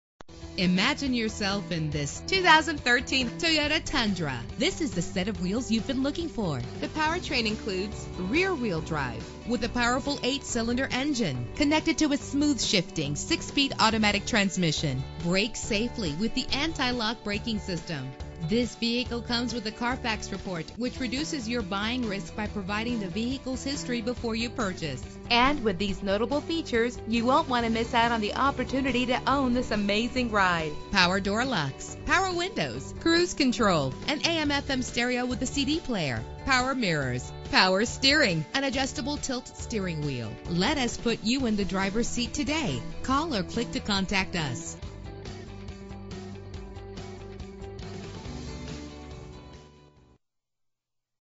2013 Toyota Tundra - Slidell LA
This vehicle represents similar vehicles available at Toyota of Slidell.
Year: 2013
Make: Toyota
Model: Tundra
Trim: Grade
Engine: 4.6 liter 8 cylinder 32 valve MPFI DOHC
Transmission: 6-Speed Automatic
Color: Brown
Mileage: 20325
Address:
300 Howze Beach Rd